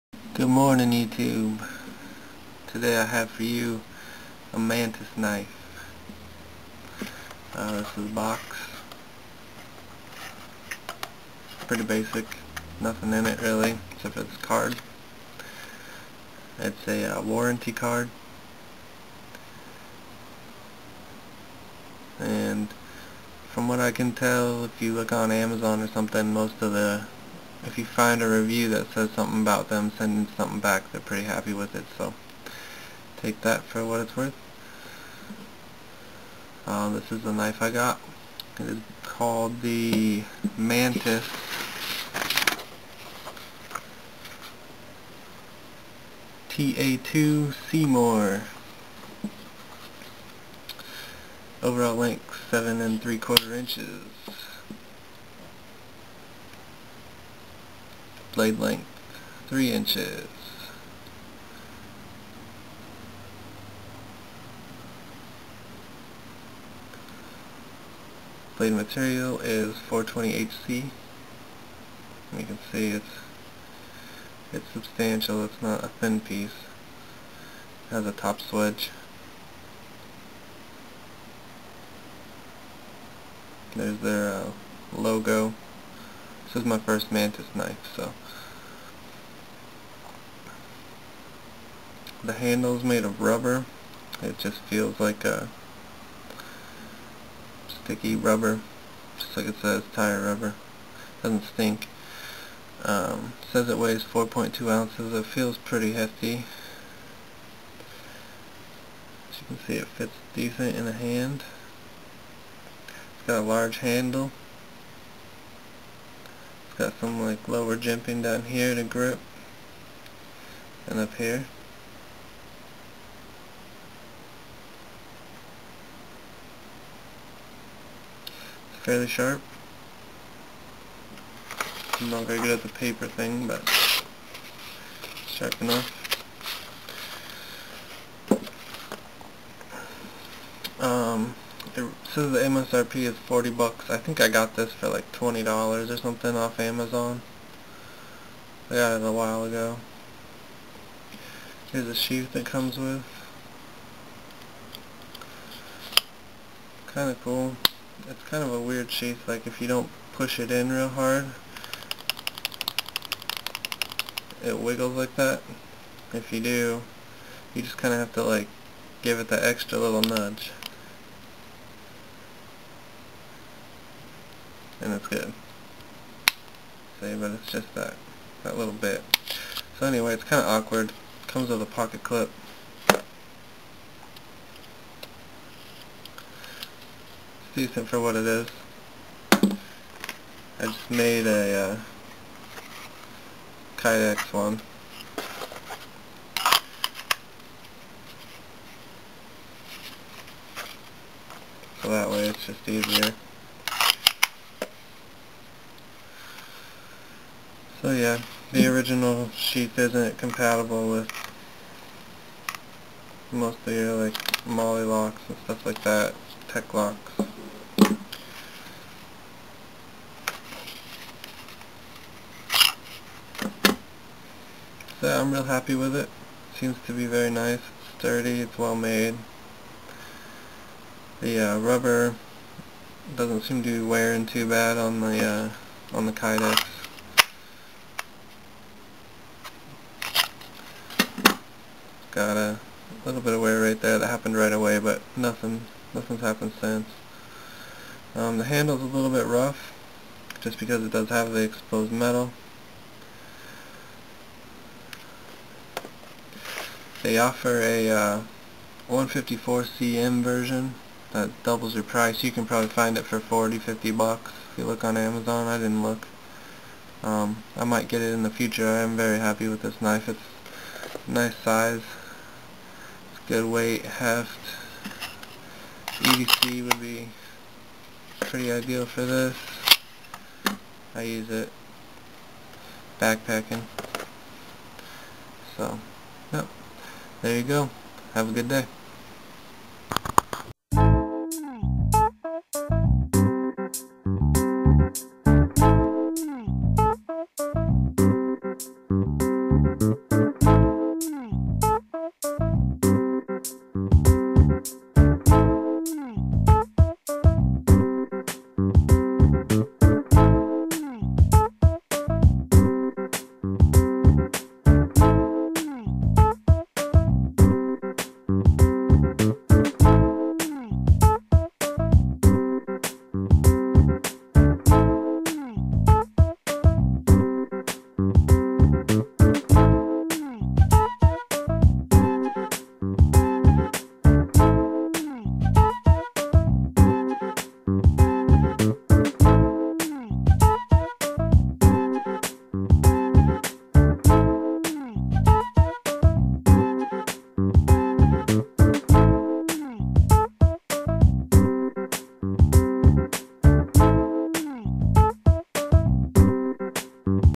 Mantis Knives TA-2 Seymour: kc*f what ya heard, I'll give them a chance
The Mantis Knives TA-2 "Seymour" knife. Though hated on the forums, I think that Mantis knives makes a decent product, and most definitely thinks outside the box when it comes to design. I plan on buying one of their "nicer" products in the near future to see if I like it as well. If you want the specs, watch the video.